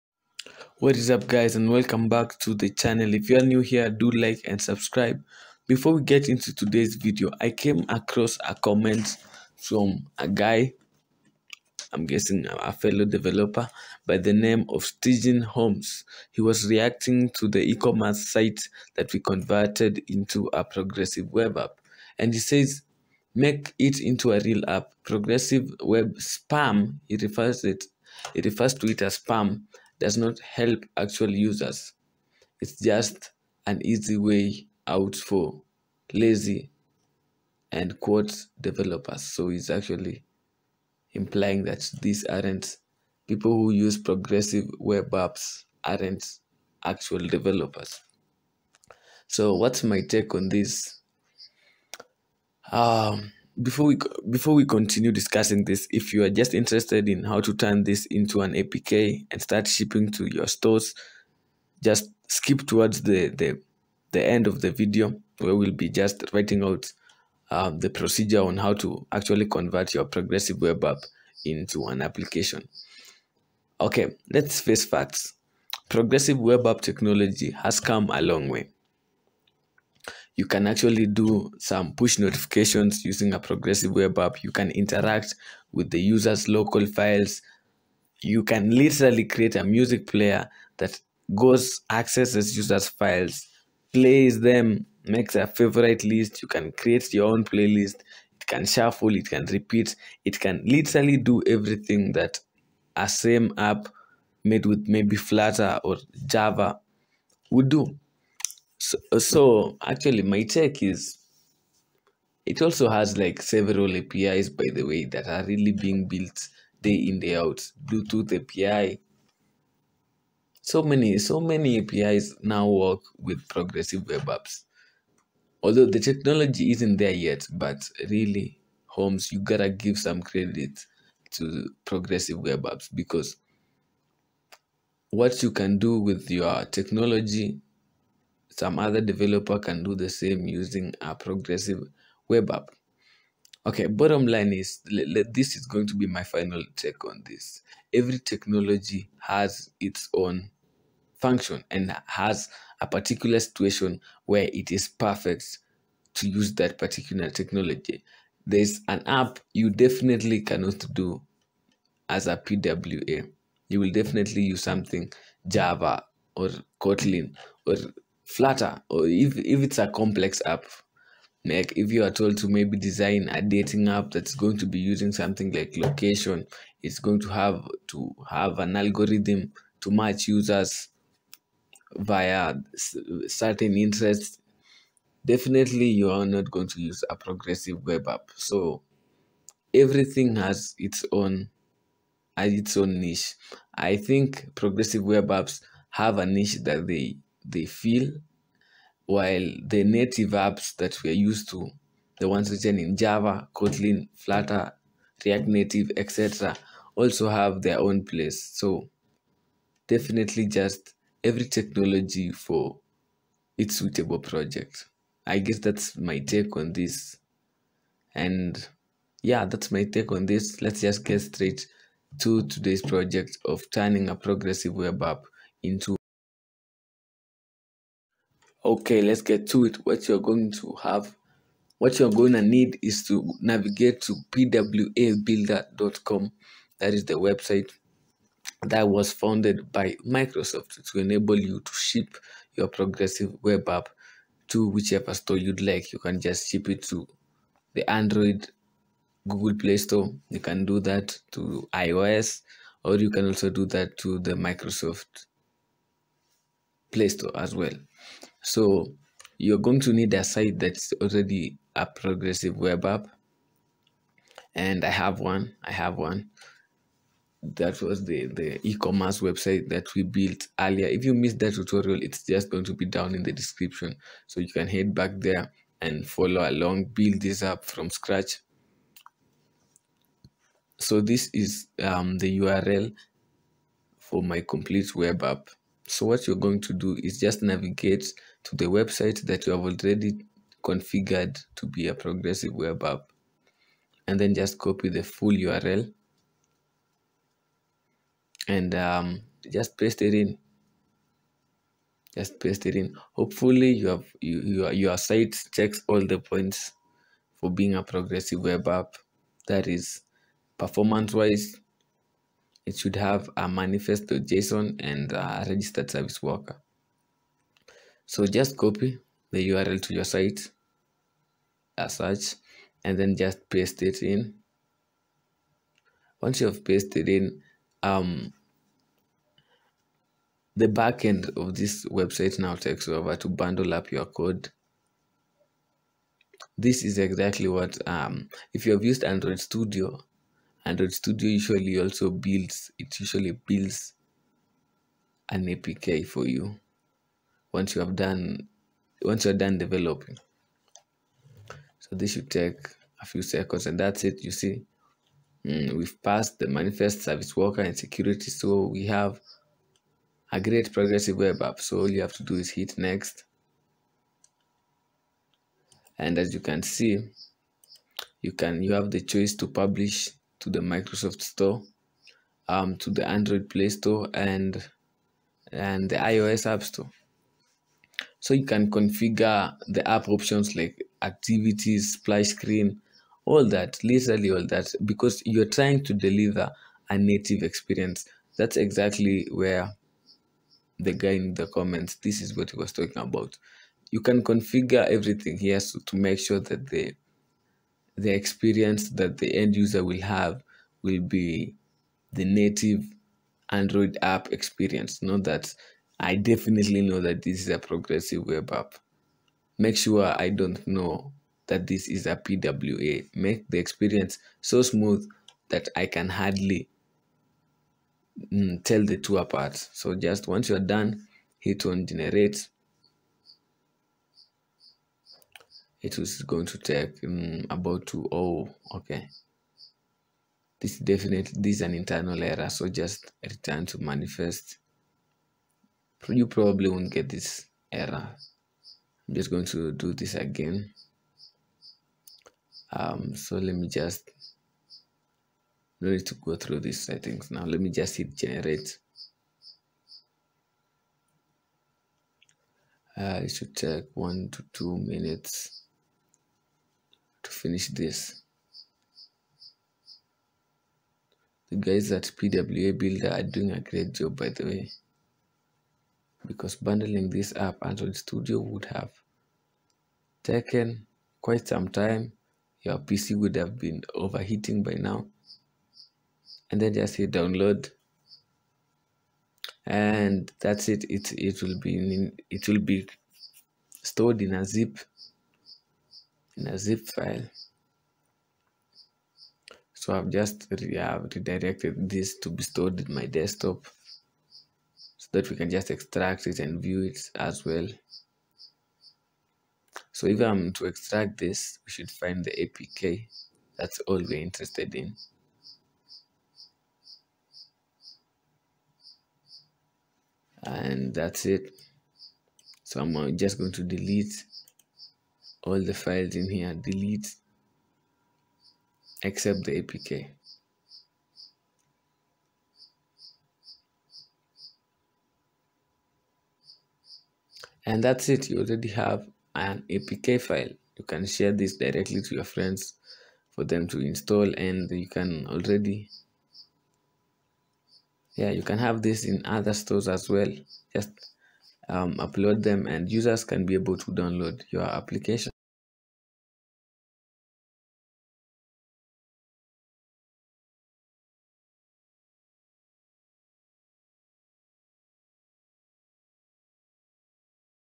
Convert a PWA into an Android App
Convert a PWA into an Android App, get an apk file that you can share and ship to the google play store or other stores.